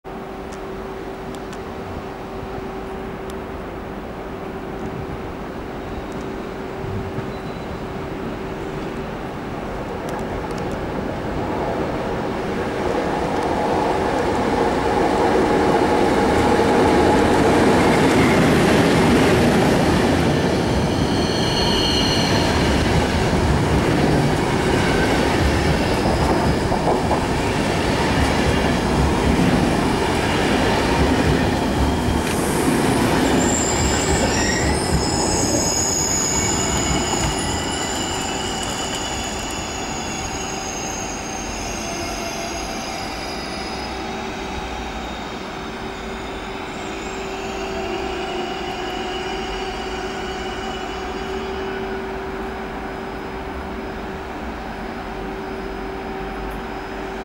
EP09-015 z EIC Sobieski rel. Wien Meidling - Warszawa Wsch. wjeżdża do Katowic.
|Katowice| EP09-015 z EIC 104 ''Sobieski'' relacji: Wien Meidling - Warszawa Wschodnia wjeżdża do stacji Katowice.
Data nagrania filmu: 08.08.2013

MasterPKP